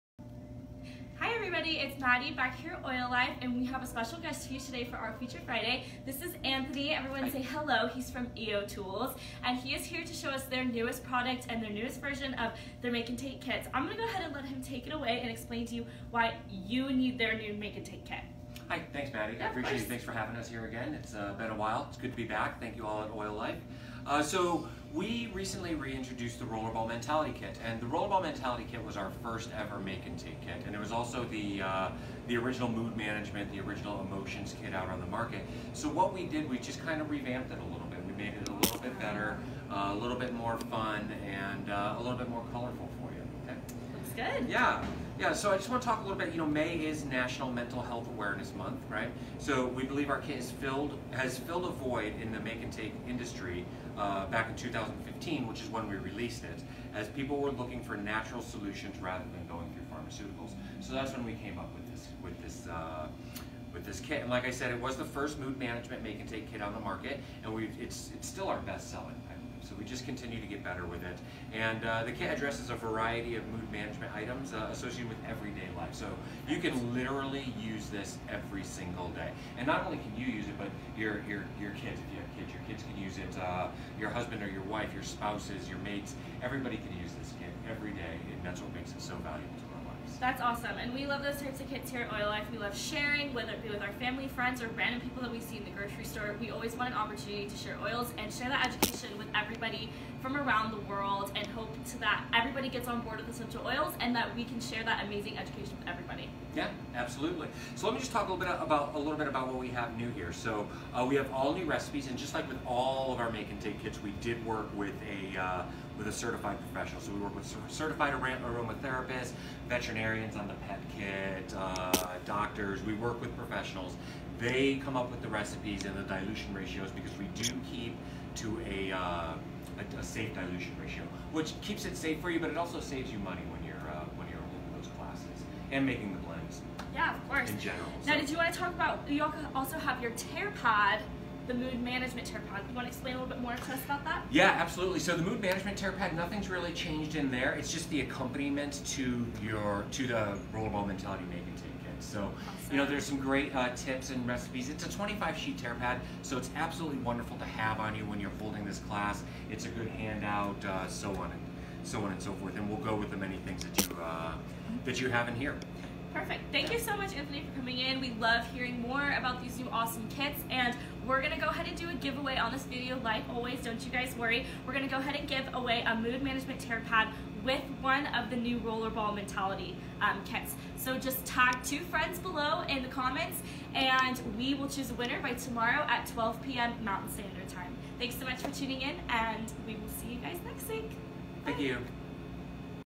Rollerball Mentality Make-and-Take Kit with EO Tools
DIY Tuesday: Rollerball Mentality Make-and-Take Kit with EO Tools!